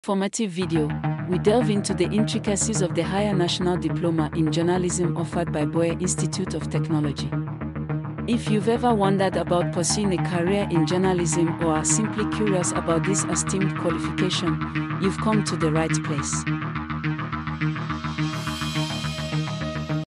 Unlock Your Potential with an HND in Journalism at Buea Institute of Technology
If you're looking to unlock your potential in the field of journalism, look no further than the Buea Institute of Technology (BIT) in Cameroon!

In this video, we'll explore how pursuing a Higher National Diploma (HND) in Journalism at BIT can help you develop the skills and knowledge you need to succeed in this exciting and dynamic field.

Led by experienced instructors with years of industry experience, the HND in Journalism program at BIT combines theoretical coursework with practical training and hands-on experience to give you a well-rounded education in journalism.

You'll learn how to gather and analyze information, write compelling stories across a range of formats, and engage with audiences through digital media and social networks. You'll also have the opportunity to work on real-world projects and gain practical experience through internships and other industry partnerships.

Whether you're interested in print, broadcast, or digital journalism, the HND in Journalism program at BIT can help you take your career to the next level. So if you're ready to unlock your potential and pursue your passion for journalism, apply to BIT today and start your journey towards a successful and fulfilling career!